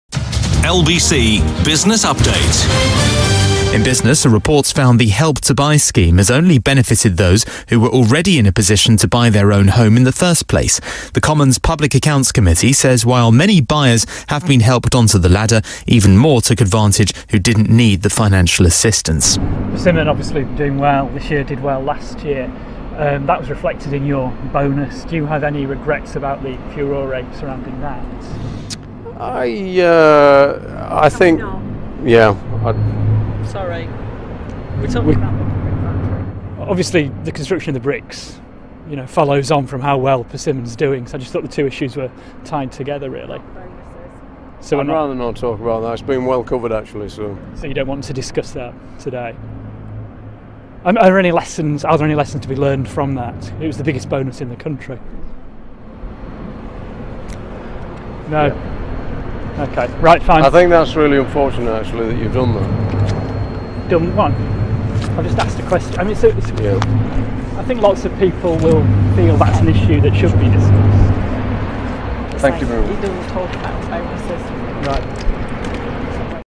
Help to Buy Profits for Builders like Persimmon Homes
A Commons Select Committee has found the value of Help to Buy uncertain.

This follows the UK’s highest paid chief executive, Jeff Fairburn of housebuilder Persimmon, who was ousted after collecting a £75m bonus.  When questioned about it he abandoned the interview.